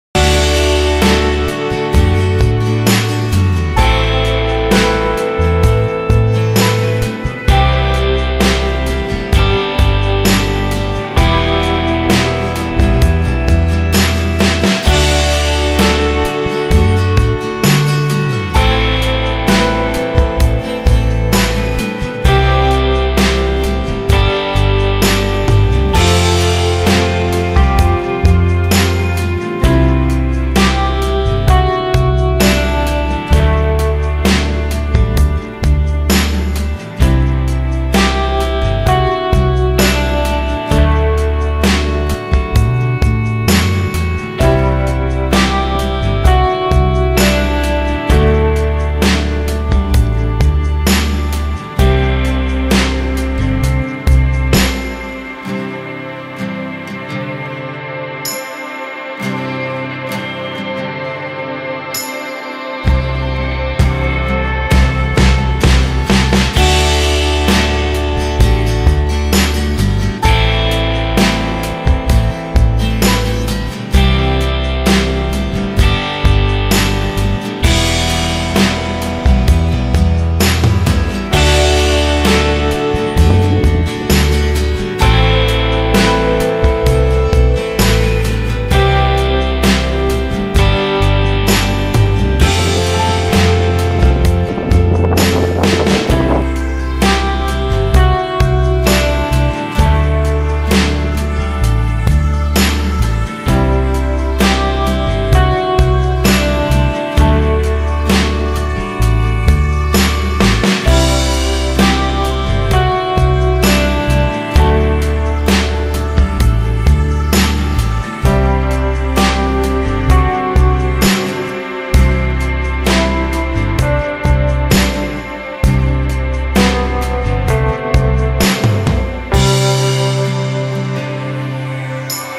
Nad Odrą cz.1 - rozpoczęcie sezonu 2016
Krótki wypad nad Odrę z feederem oraz test granulatu z ziaren jako zanęty do koszyka.W dwie godziny 8 rybek złowionych i kilka nie zaciętych brań:)